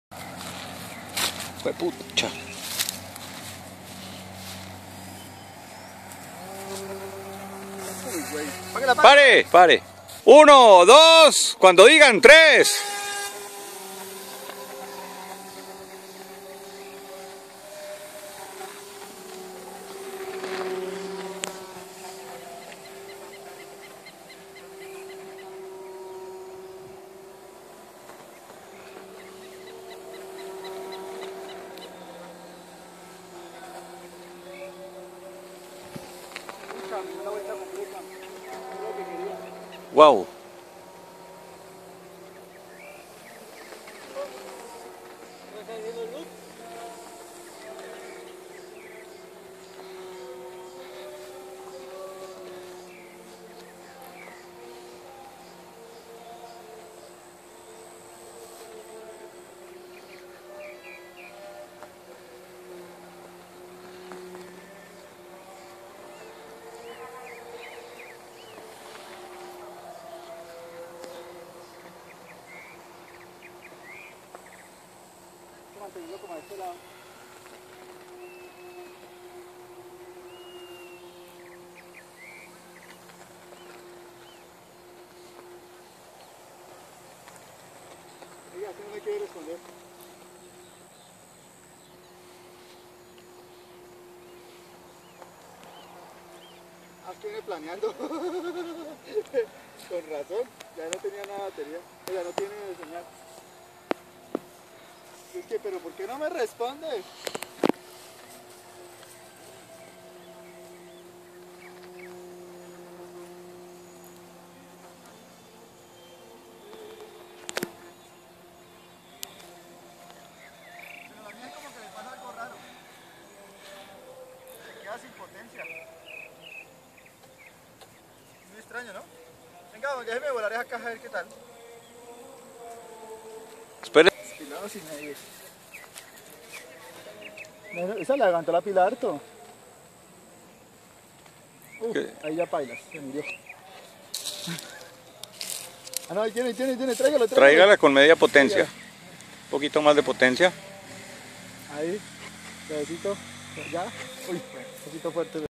Cometas RC Bogotá Colombia
Cometa tipo delta en ripstop poliester y estructura en fibra de vidrio con un sistema para manejo RC.
Vuelo de cometa delta en un gran campo despejado en Villavicencio con unos Amigos.